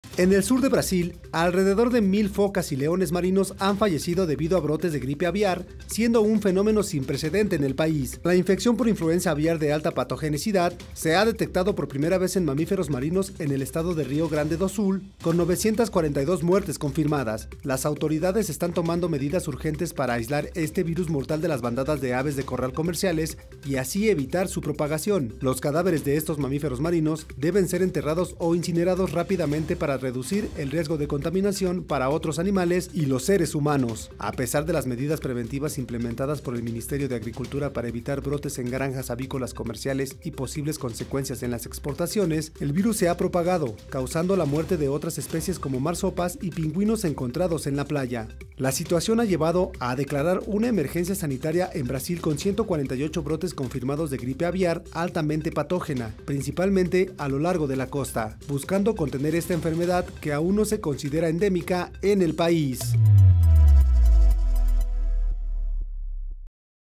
Brote de gripe aviar mata a más de 900 focas y leones marinos en el sur de Brasil
Los científicos también han encontrado algunos mamíferos marinos convulsionando en las playas locales, ya que el virus ataca su sistema nervioso.